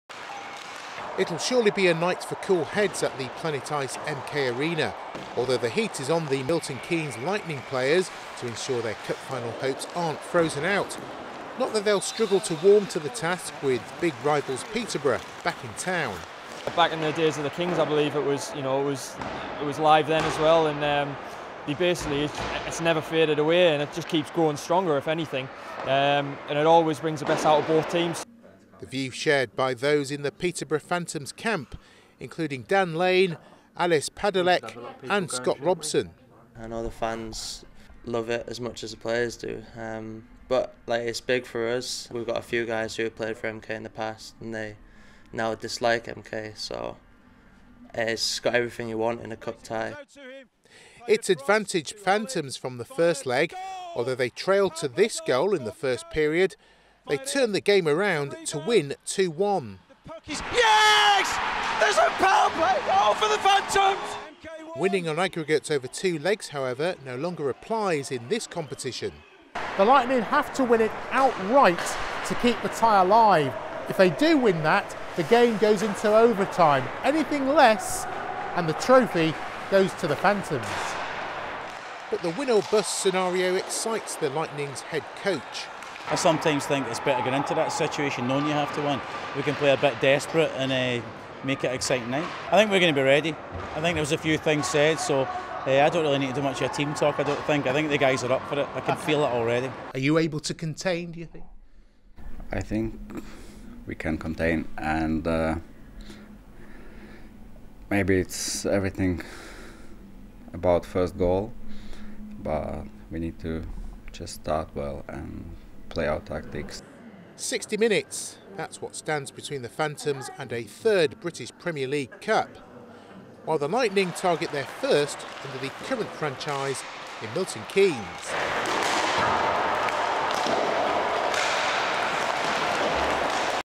Pre-Cup Final Interview with ITV Anglia
Interview with ITV Anglia prior to the English Premier League Cup Final between MK Lightning and Peterborough Phantoms.